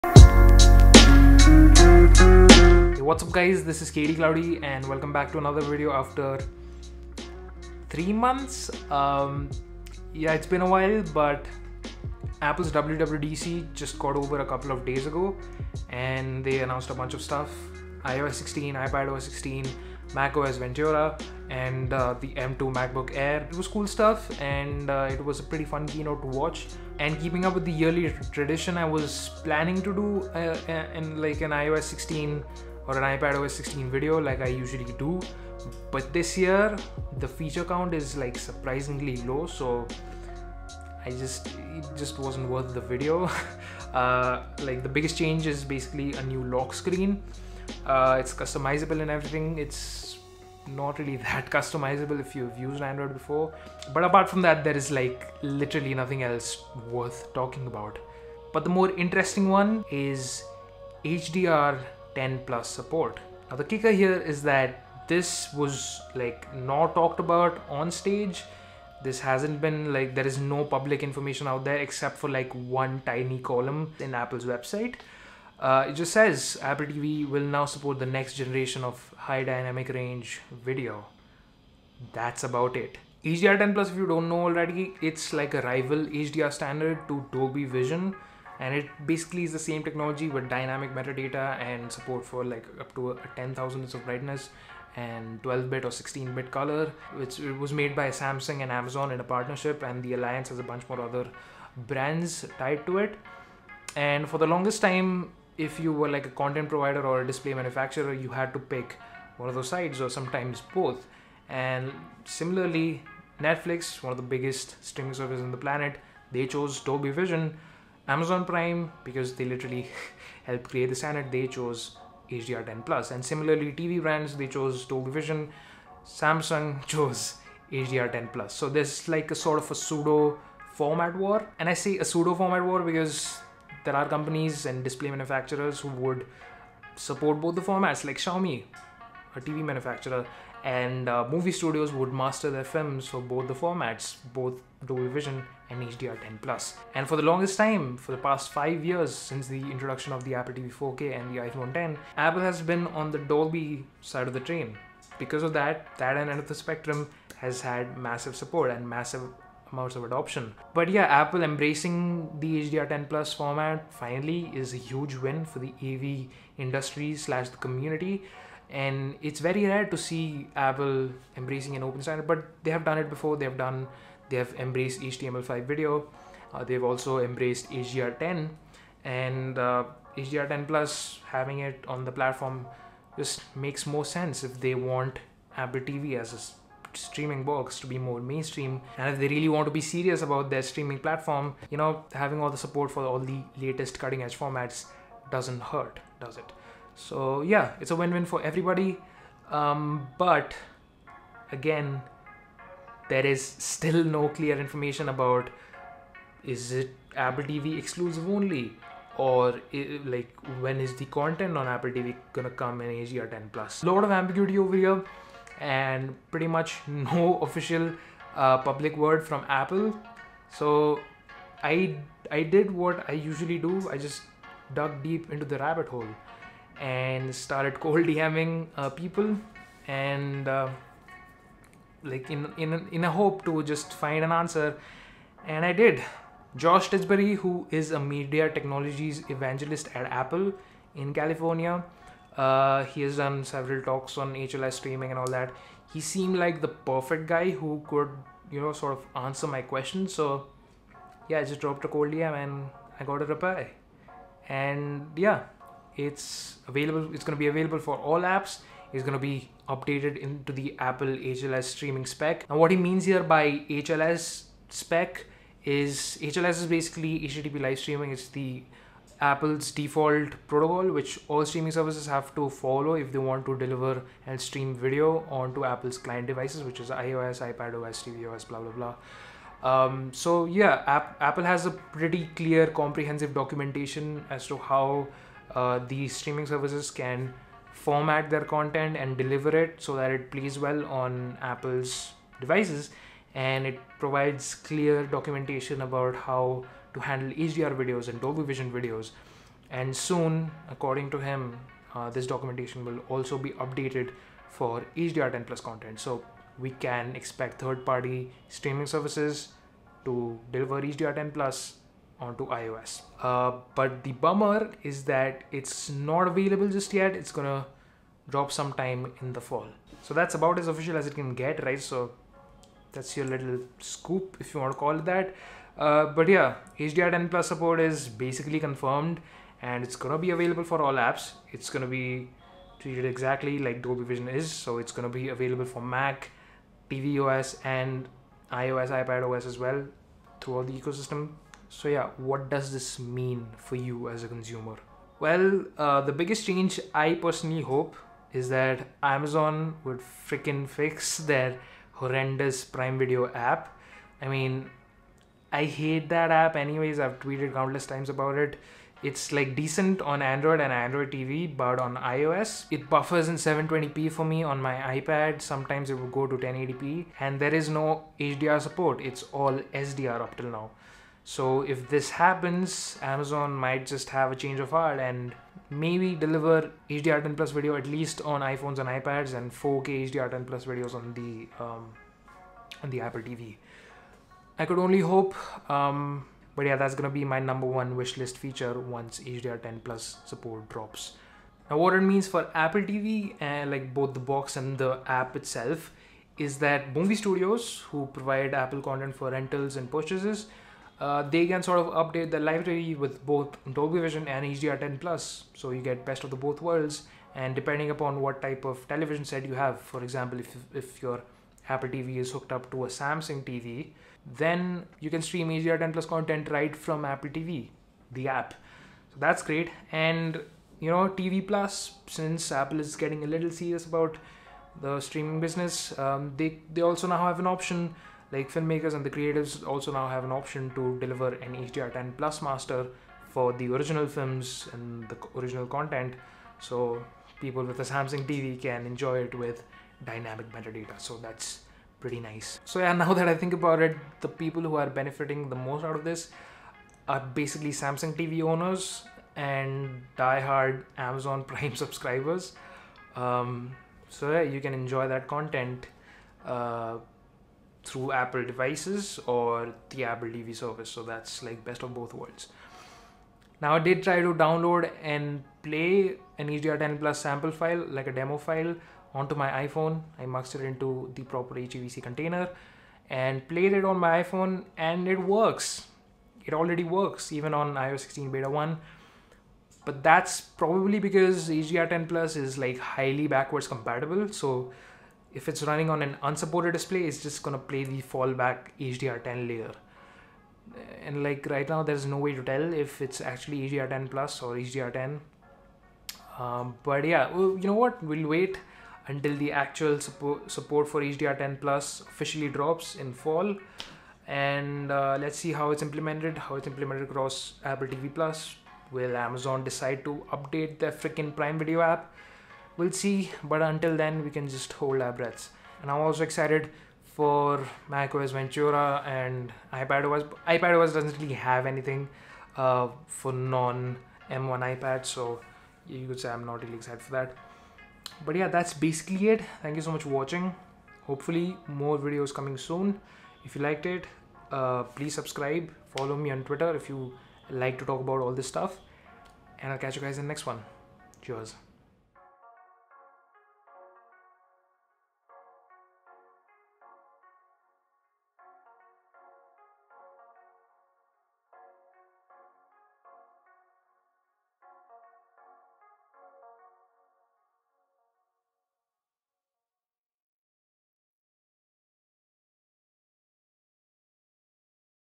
HDR10+ coming to Apple TV?
wwdc22 just got over and Apple announced the latest features of iOS 16, macOS Ventura and iPadOS 16. But an announcement which went under the radar was the addition of HDR10+ support in all of Apple's platforms. Apple has always pushed HDR10, HDR and Dolby Vision and Atmos formats in all their operating systems, and the addition of a more open platform like HDR10+ definitely is a welcome surprise.